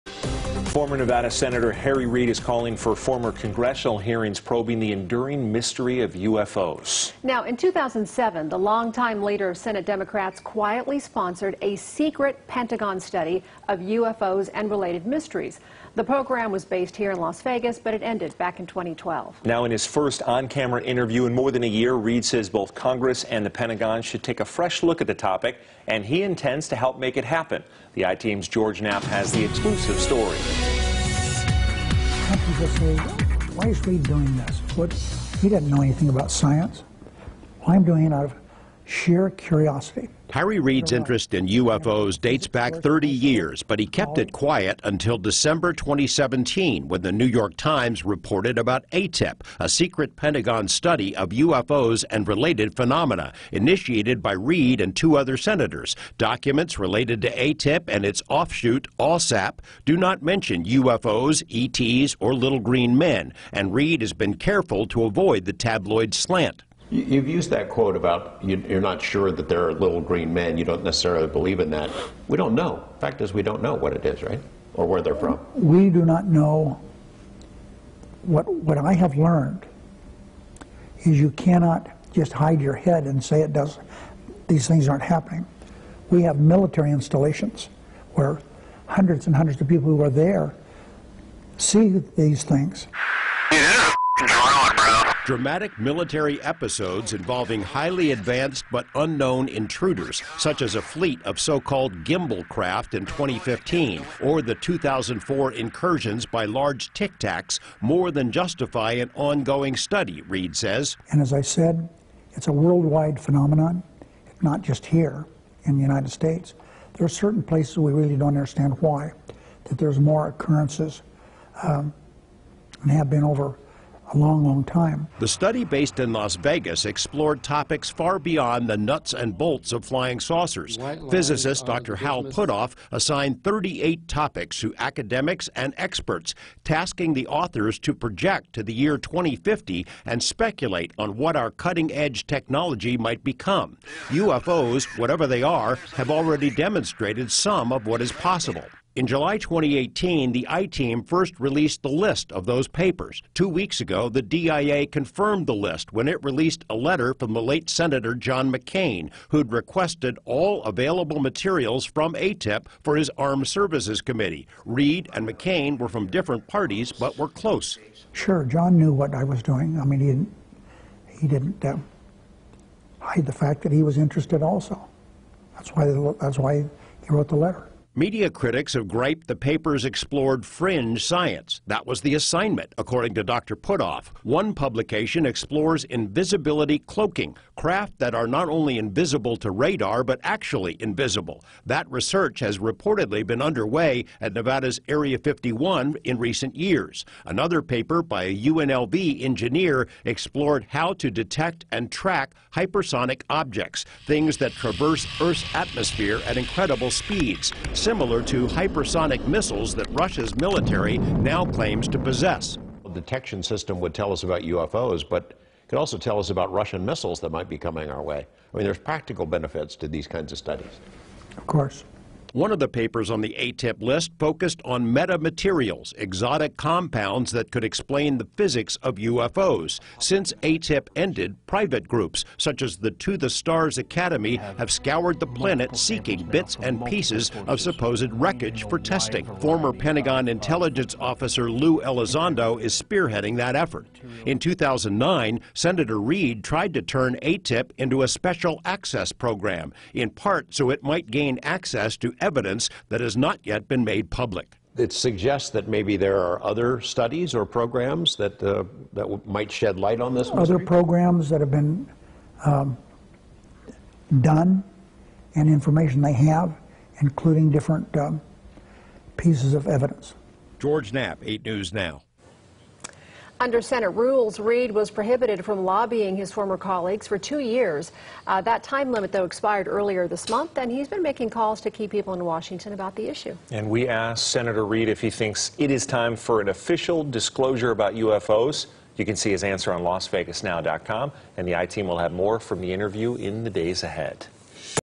I-Team: Former Sen. Reid calls for congressional hearings into UFOs
Former Nevada Senator Harry Reid is calling for formal congressional hearings probing the enduring mystery of UFOs.